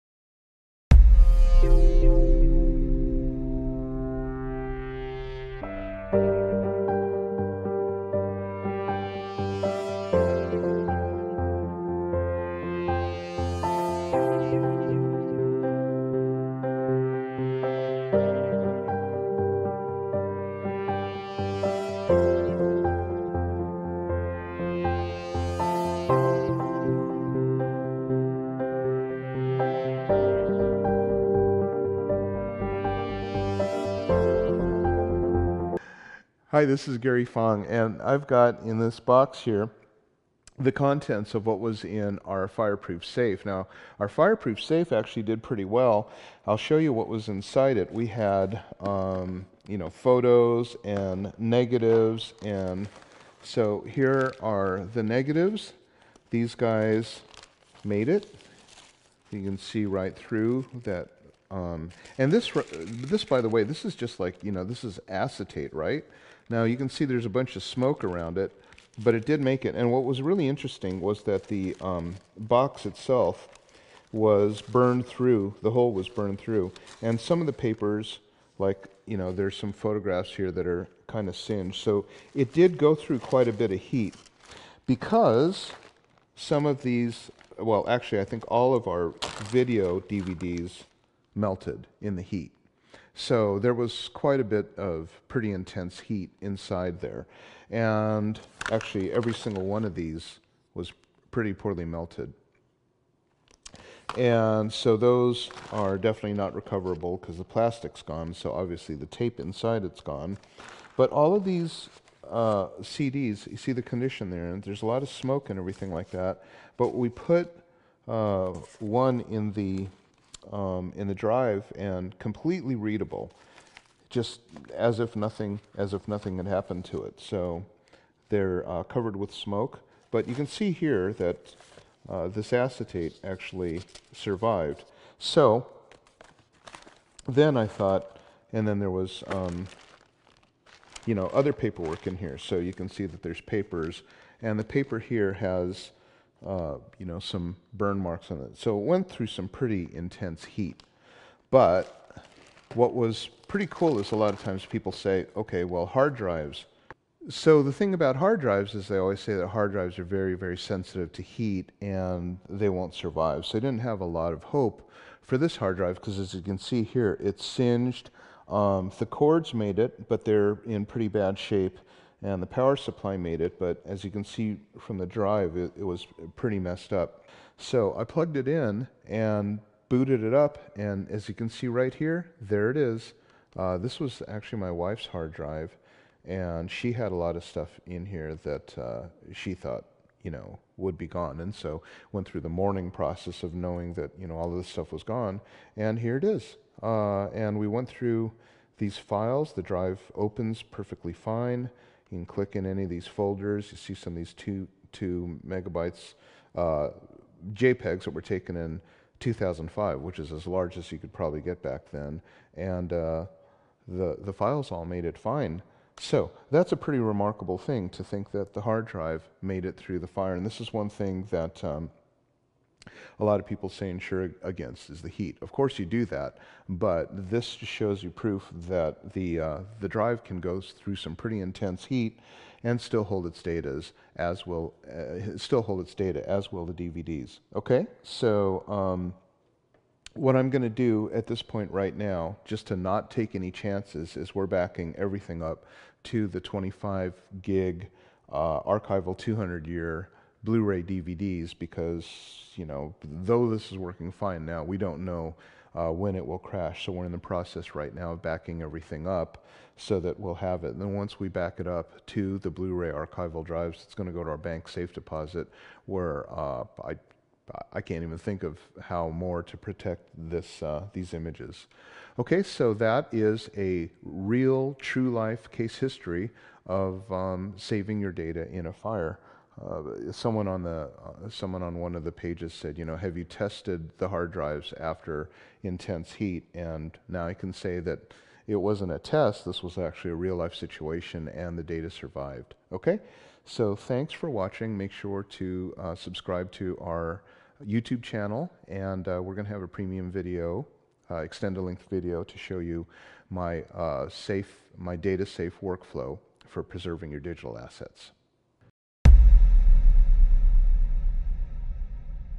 Hard Drive Survives Intense House Fire
In this video, Gary Fong boots up a backup hard drive that was left in his house which burnt completely to the ground.  It was a four day inferno that had heat so high that it melted aluminum and steel, and burned a hole through the top of the safe.  However, the contents of the drive fared well - papers, currency, negatives, Data CD's and a 250G hard drive all were in good condition.  Much of this is due to the fact that the safe was placed right next to a fireplace - the other safe in the house (on the 2nd floor) was gone completely.